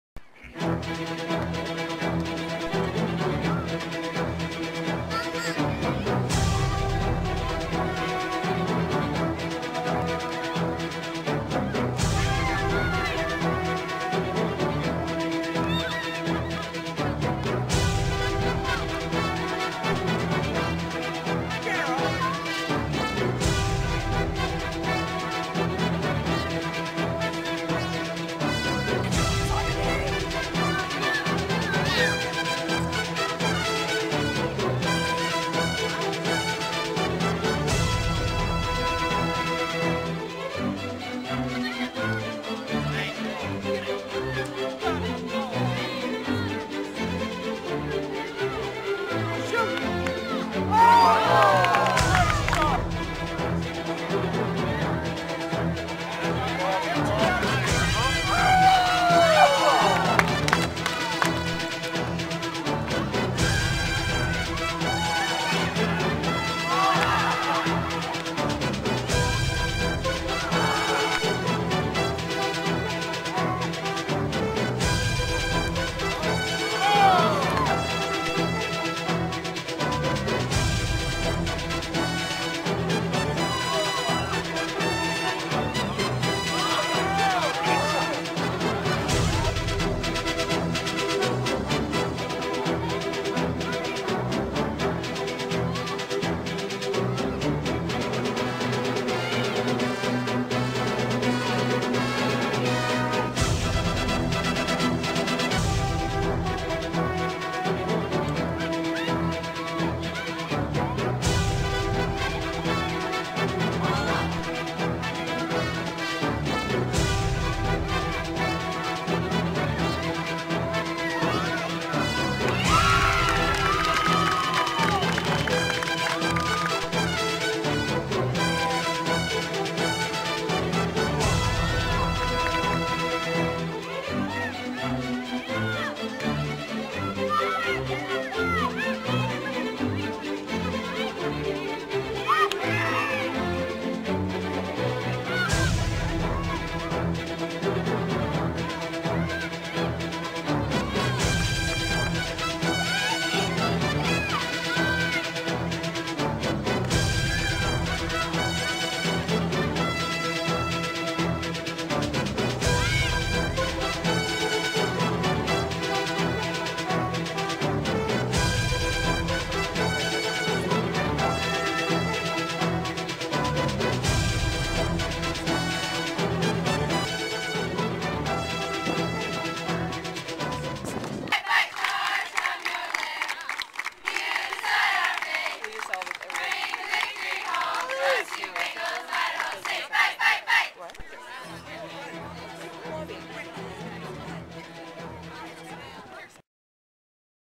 Idaho State Alumni Soccer Game 2010 Highlight Video
Idaho State ended the 2010 spring season with its annual Alumni Game at Davis Field on Saturday. The Bengals clad in white overtook the Alumni, in orange, in a 2-0 victory with both goals scored in the second period. Freshman Michelle Buhrle scored the game-winning goal, featured in this highlight video, while freshman Rachel Strawn scored the final goal, her second of the spring season.